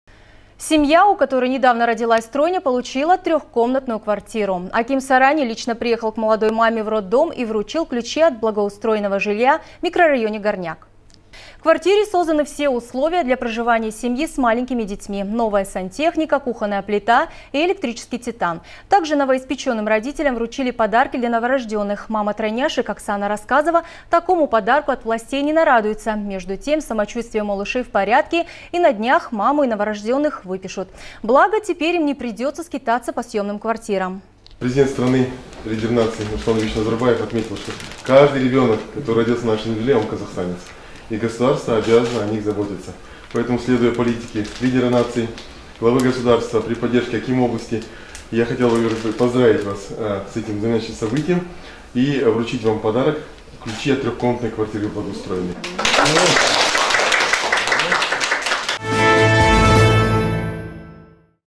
Семья, у которой недавно родилась тройня, получила трехкомнатную квартиру.
Аким Сарани лично приехал к молодой маме в роддом и вручил ключи от благоустроенного жилья в микрорайоне «Горняк». В квартире созданы все условия для проживания семьи с маленькими детьми: новая сантехника, кухонная плита и электрический титан. Также новоиспеченным родителям вручили подарки для новорожденных.